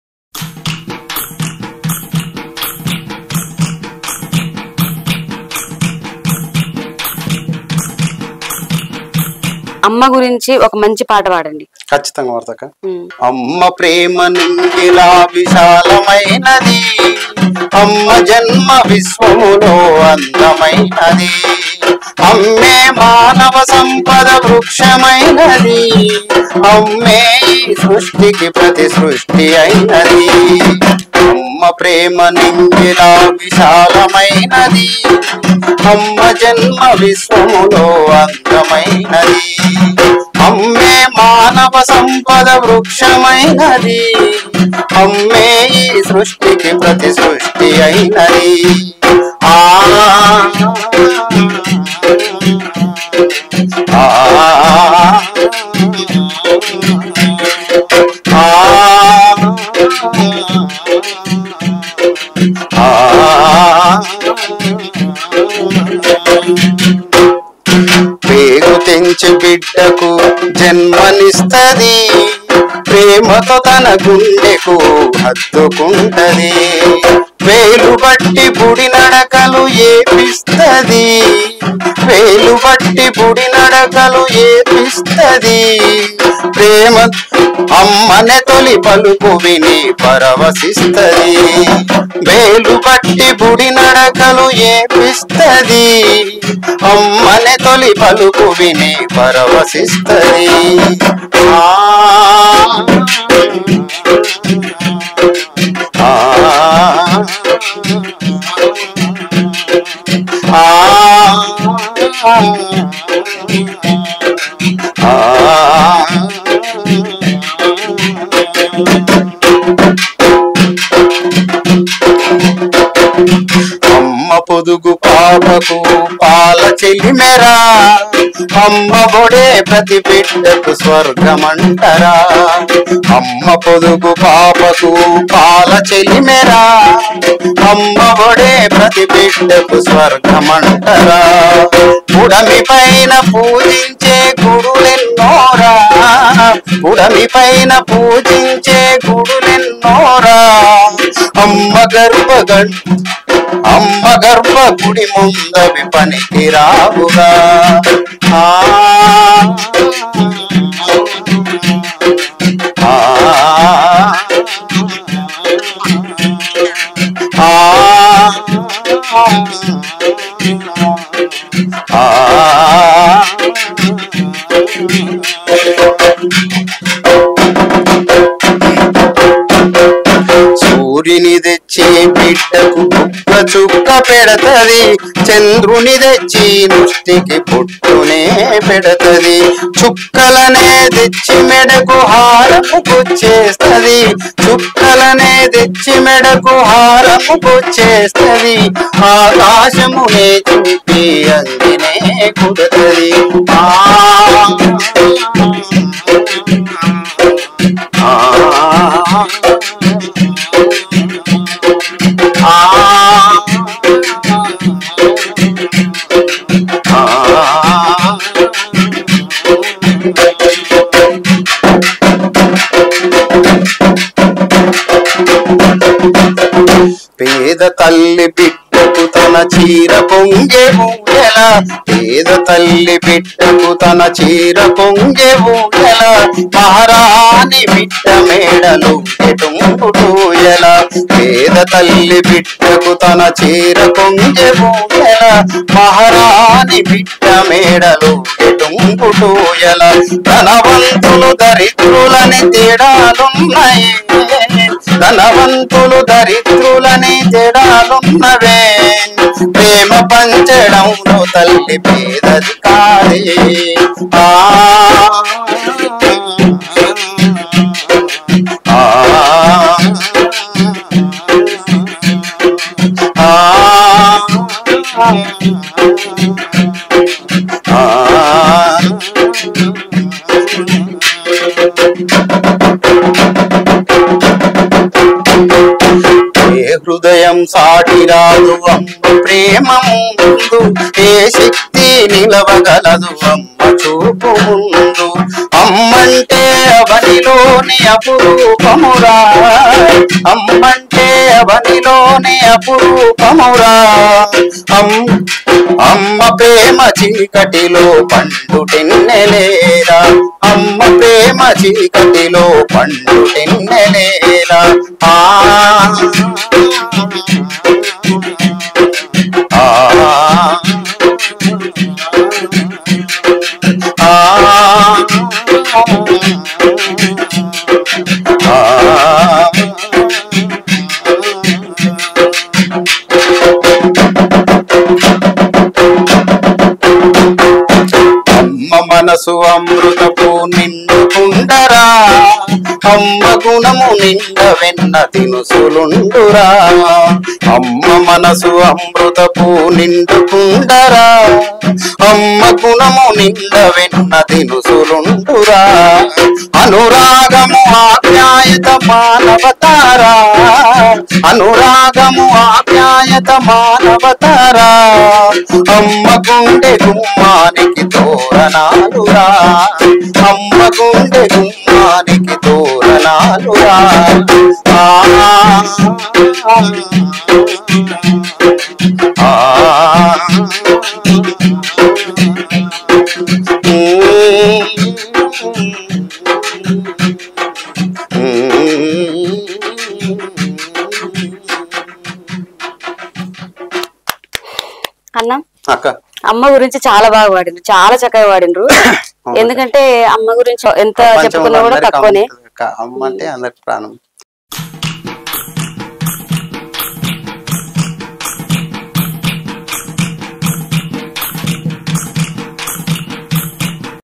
ఇప్పటివరకు వినని అమ్మపాట.. | Manku Kishan | Emotional Song on Mother | Patala bandi
Patala Bandi is purely Telugu Songs Channel. Telangana Songs. Telugu Best Singers Interviews. Telangana Folk Songs. Telangana Cultural Songs.